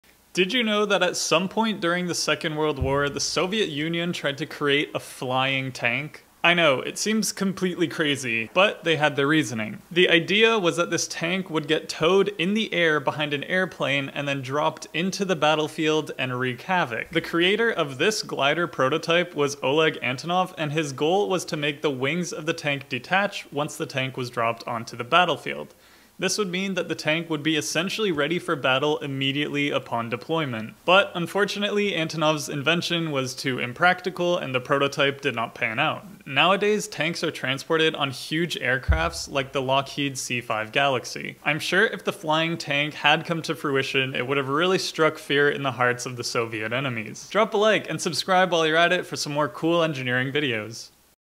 A Tank With Wings? Failed Military Engineering
The soviet union created the Antonov T-40, a prototype for a flying tank. Pretty crazy stuff right, strike fear in the hearts of their enemies.